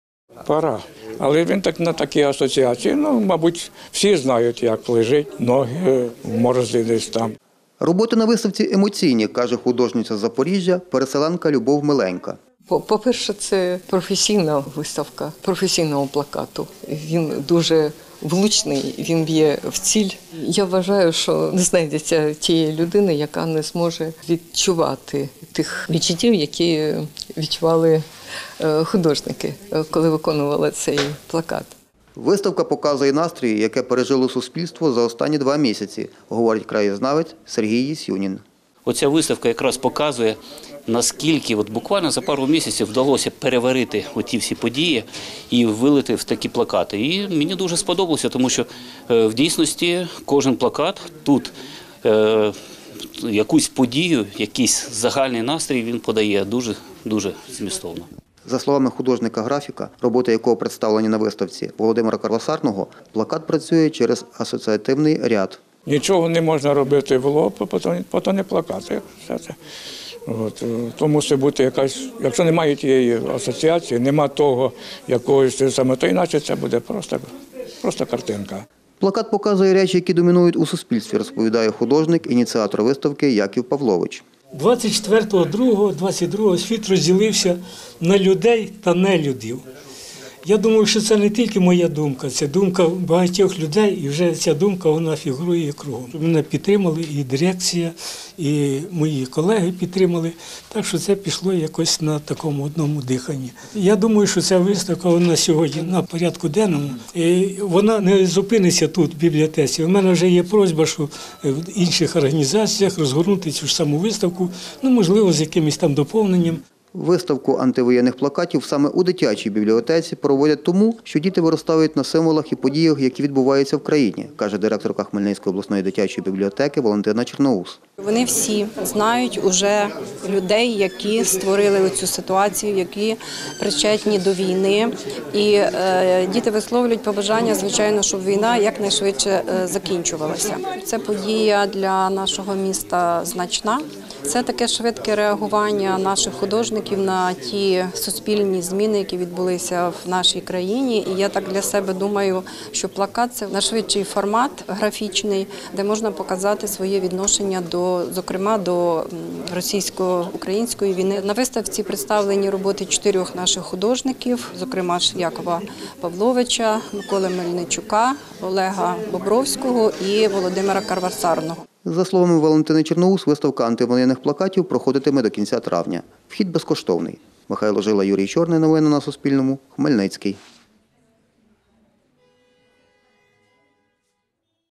24 плакати представили на відкритті виставки антивоєнних плакатів під назвою «Світ переможе темряву»
24 плакати представили на відкритті виставки антивоєнних плакатів під назвою «Світ переможе темряву» у дитячій бібліотеці. Про це розповіла директорка хмельницької обласної дитячої бібліотеки Валентина Черноус. За її словами, у виставці взяли участь 4 художники. Які плакати виставлялися на загал, враження відвідувачів, та які емоції відчували самі художники при створенні робіт далі у сюжеті Суспільного.